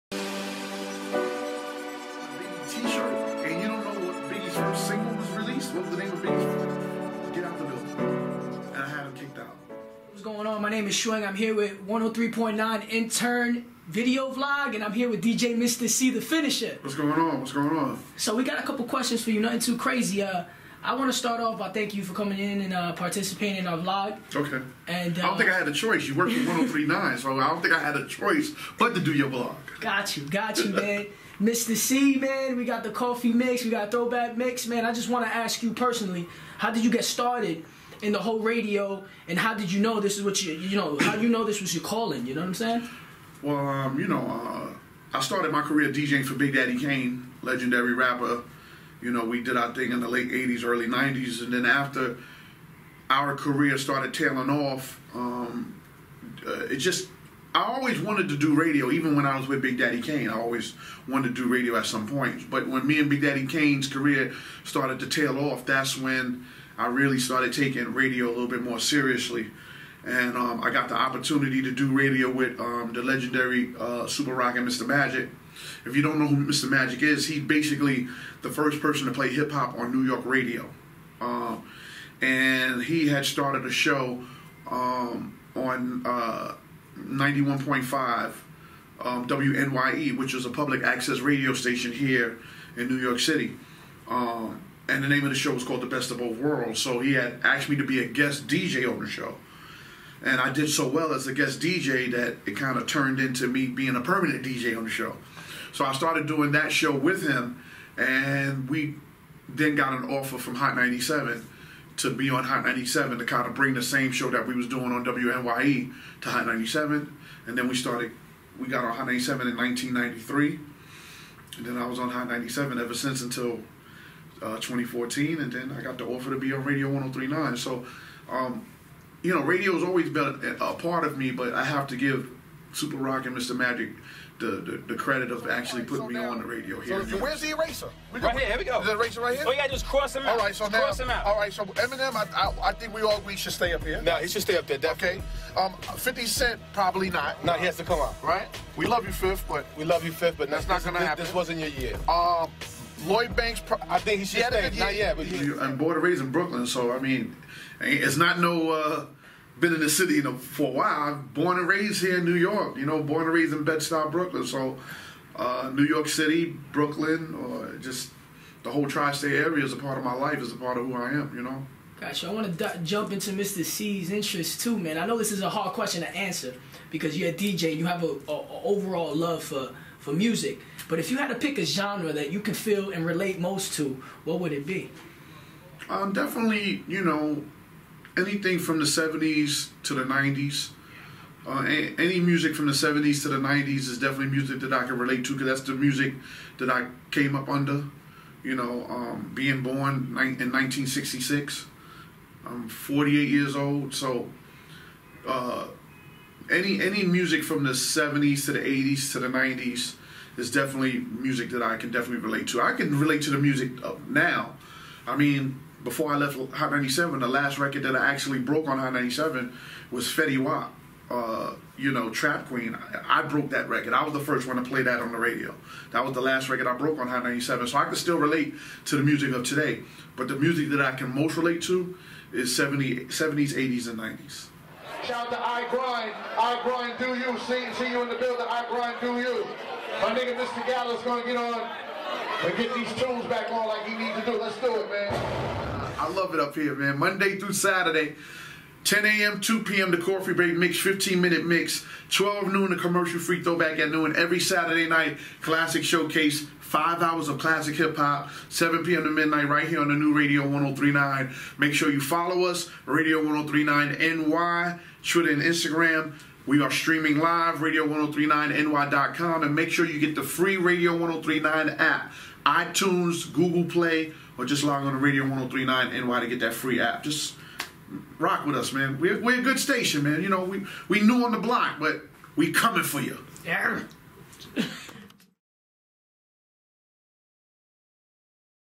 Radio 1039 NY's Intern Video Vlog Interview with Dj Mister Cee
Radio 103.9 NY's sits down with legendary american hip hop dj & Radio personality. "Get to know DJ Mister Cee"